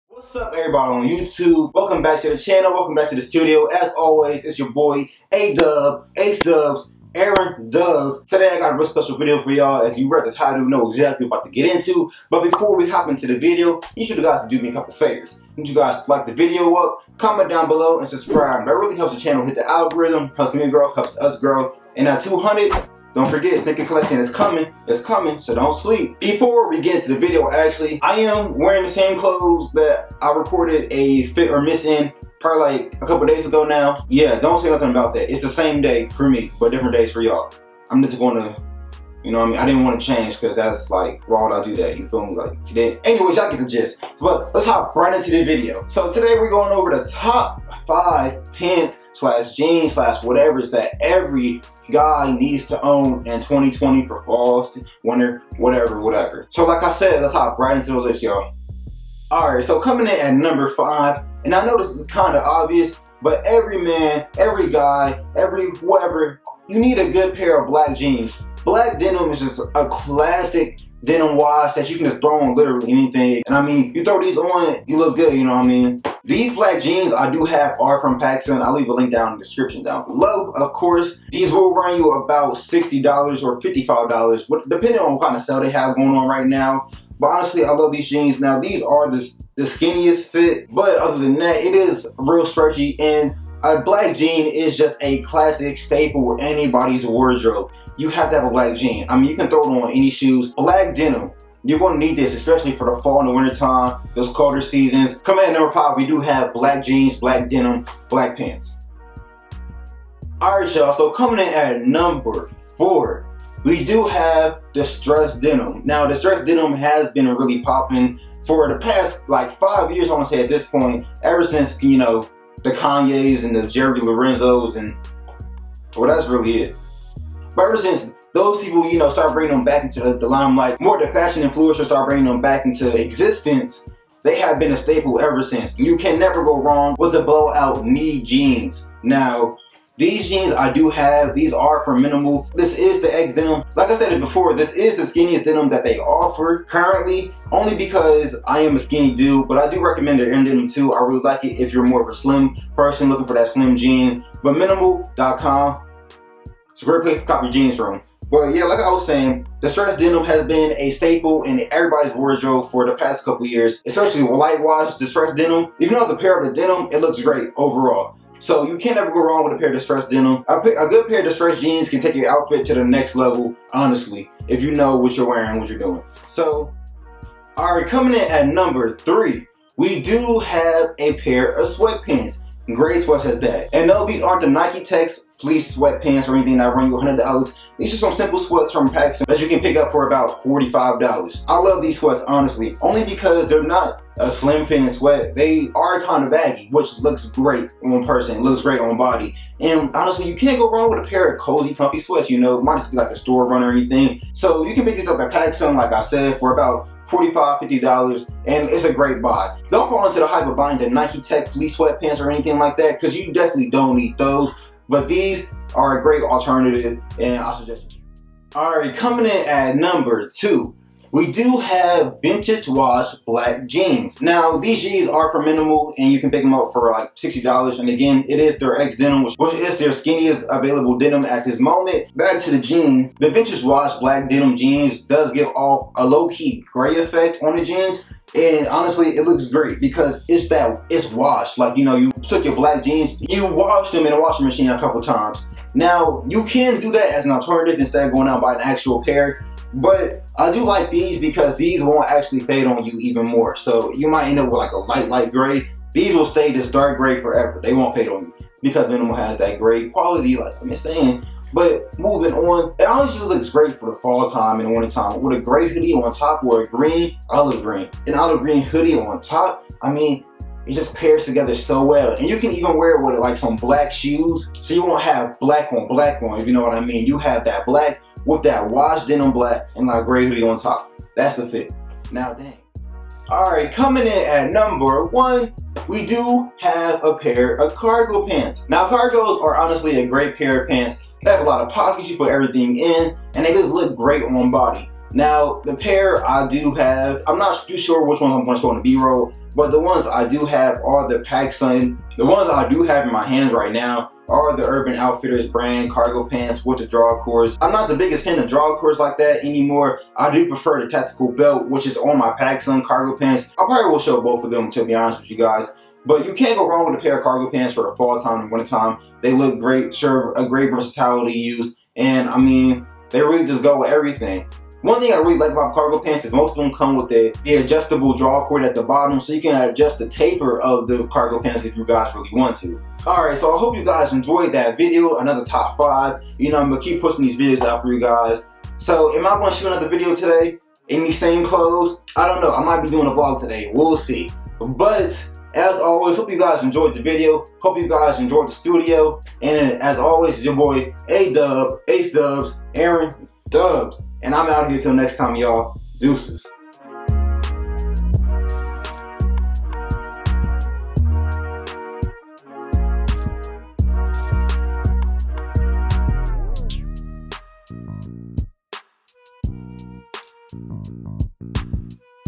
TOP 5 PANTS EVERY GUY SHOULD OWN/ FOR BACK TO SCHOOL
In this video ADubs goes over the top 5 pants that every guy should own, as well as the Top 5 pants for back to school. These pants are great for hypebeast as well just regular people trying to bring their outfit to the next level.

Cop the pants here!

~SONGS~
All songs and beats are owned by Gavin Clarke! Go support him and check out his stuff!